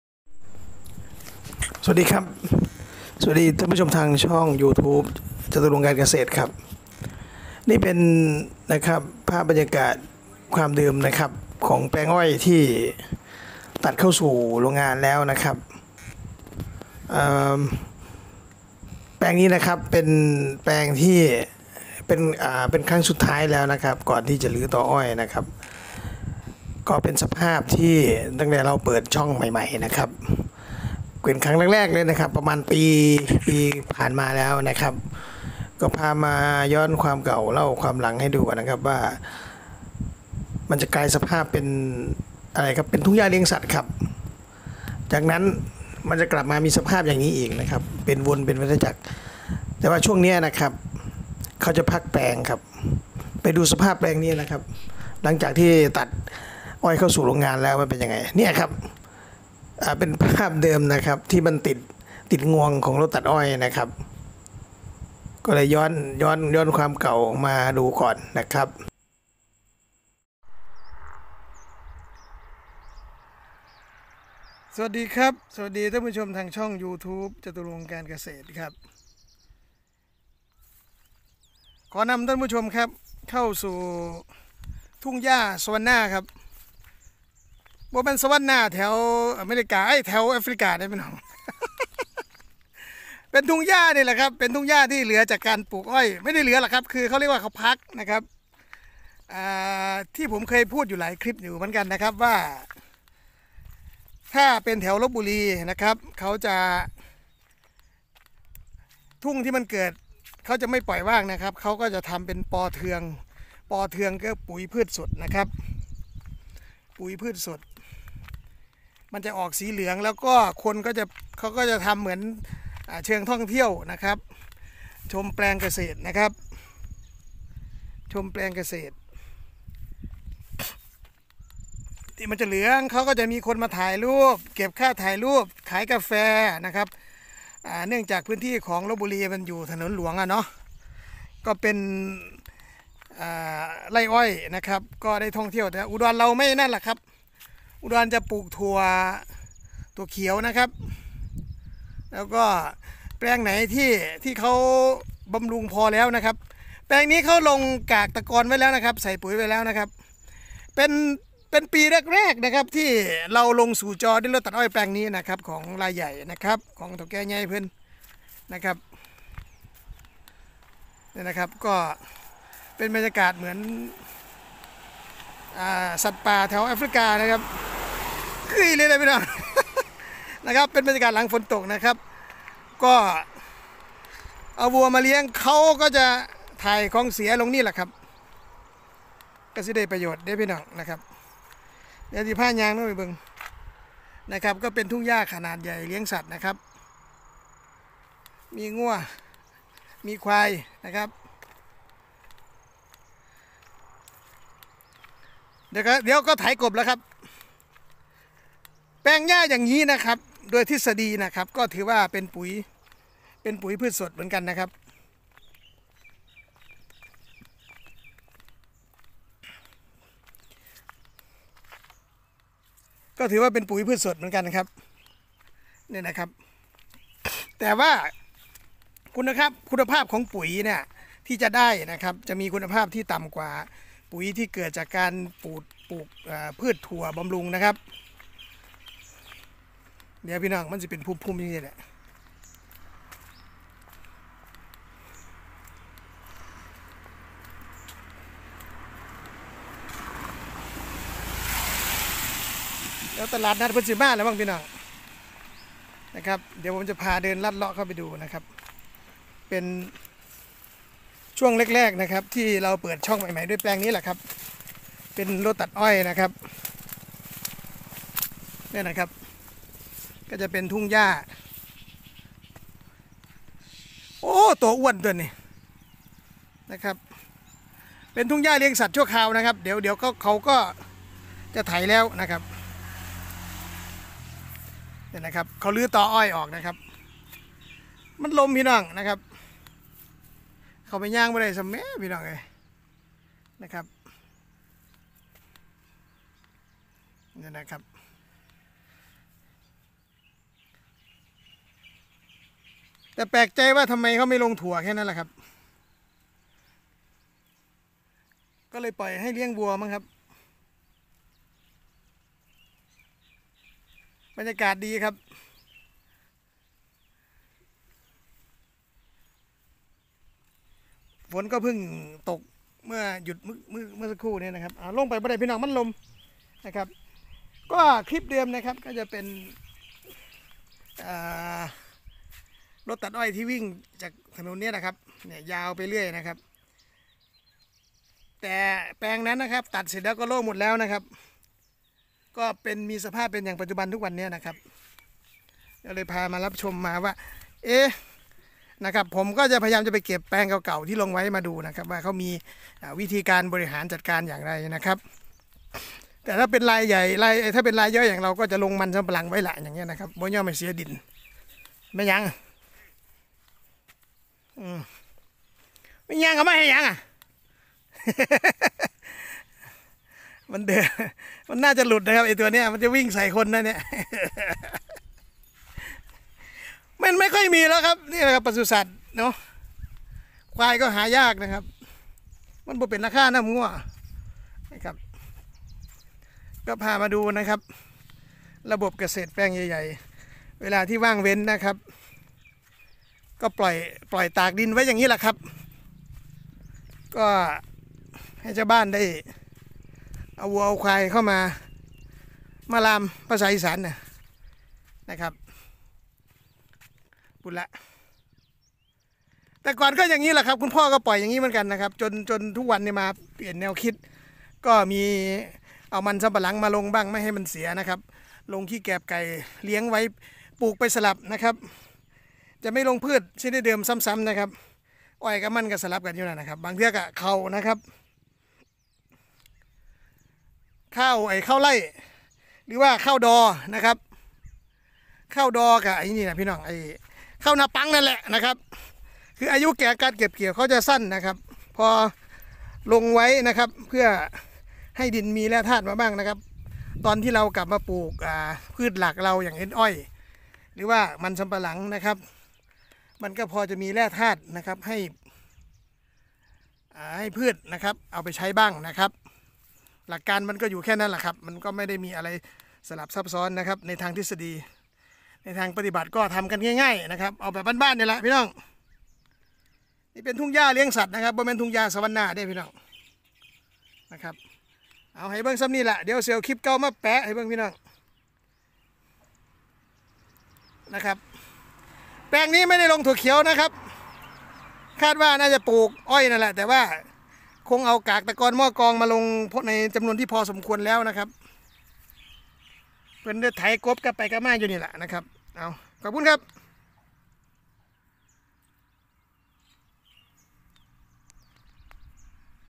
จากทุ่งหญ้าเลี้ยงสัตว์จะกลายสภาพเป็นแปลงอ้อย
คลิปนี้จะพาไปรับชมทุ่งหญ้าเลี้ยงสัตว์ที่เกิดจากการพักแปลงอ้อย จากนั้นจะกลายสภาพเป็นแปลงอ้อยที่สมบูรณ์ต่อไป